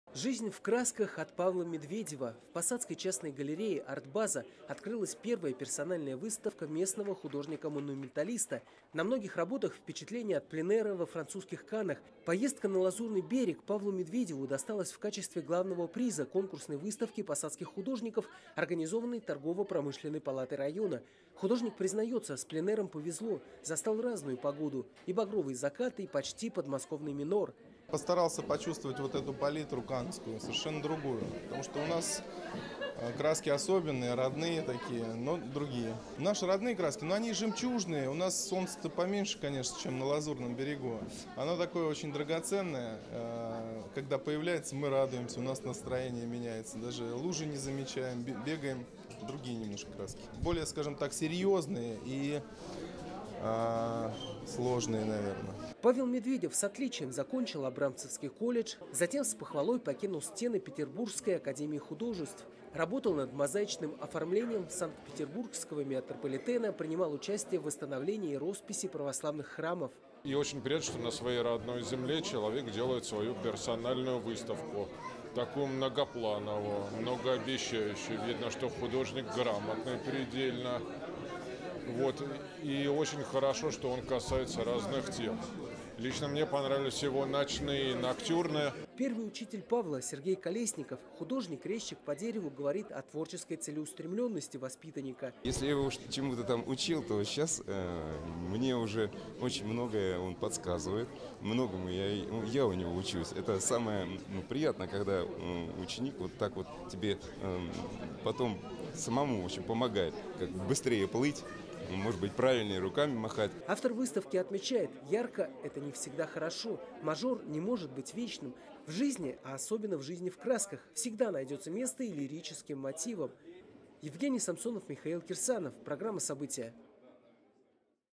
Павел Медведев отчитался о пленэре в Каннах
17 марта 2017 | «Жизнь в красках» от Павла Медведева. В сергиевопосадской частной галерее «Арт-База» открылась первая персональная выставка местного художника-монументалиста. На многих работах – впечатления от пленэра во французских Каннах.